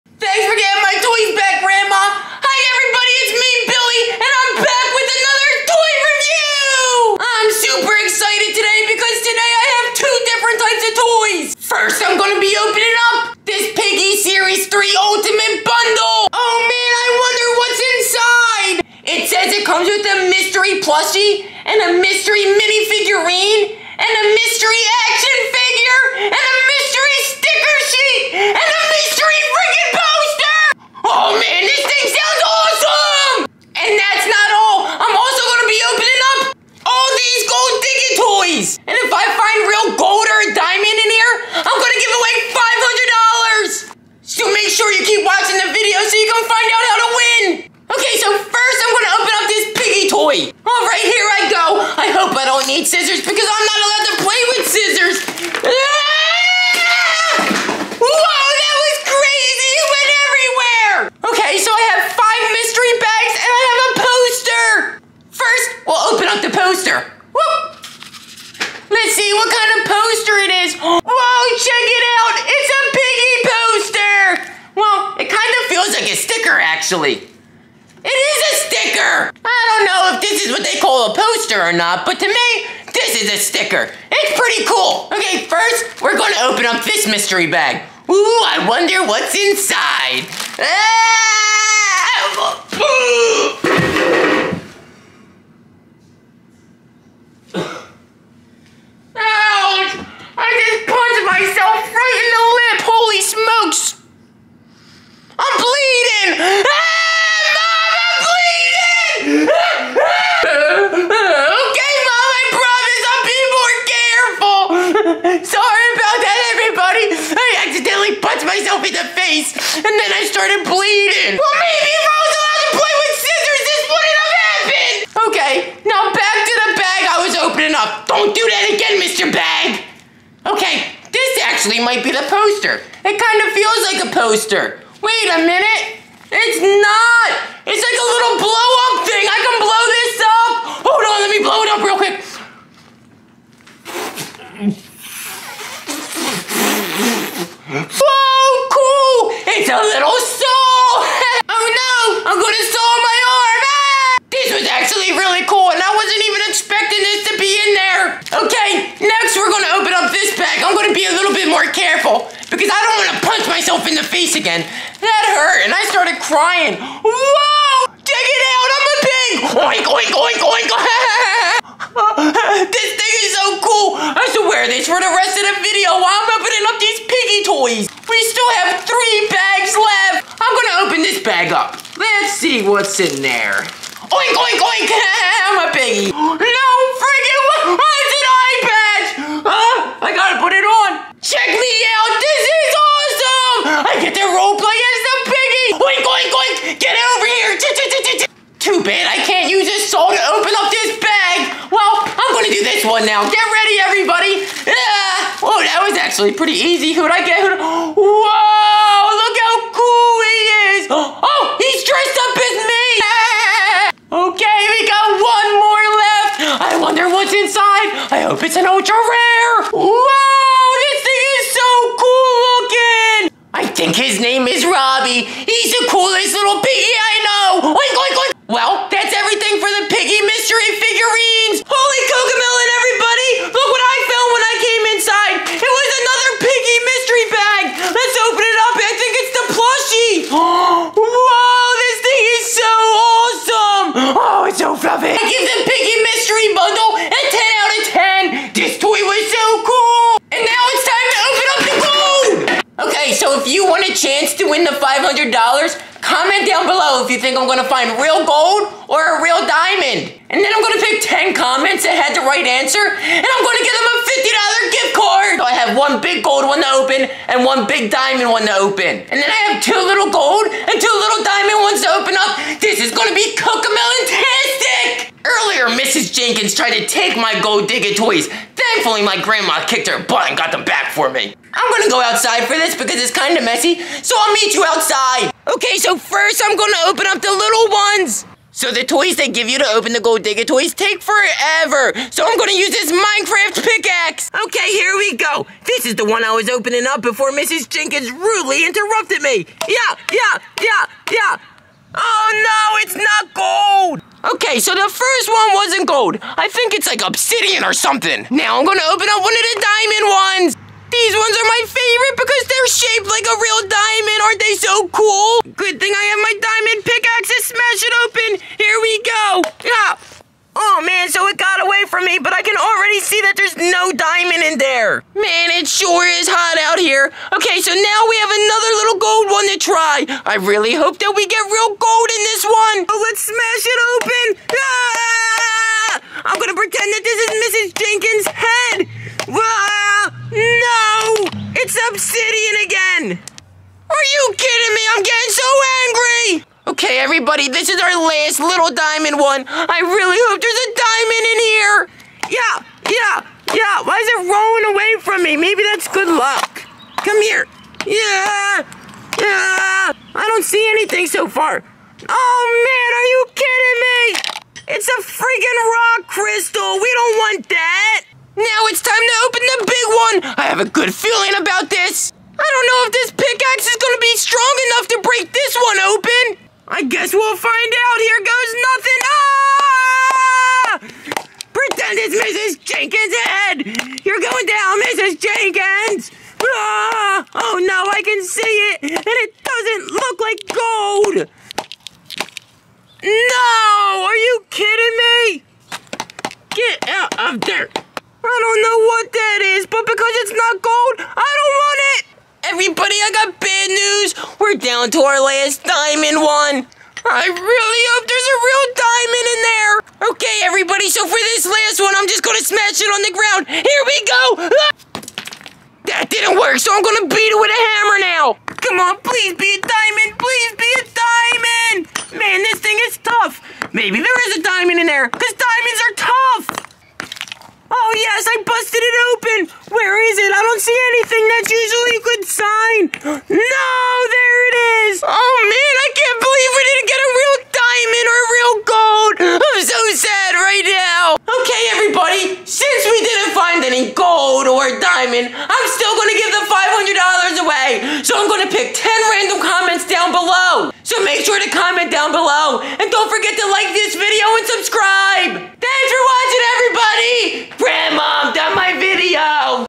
Billy's Toys Review (Living With Siblings)Giving away 500$
In this episode of billys toys review Billy opens up. Piggy Series 3 Ultimate bundle, And Gold Dig it Toys.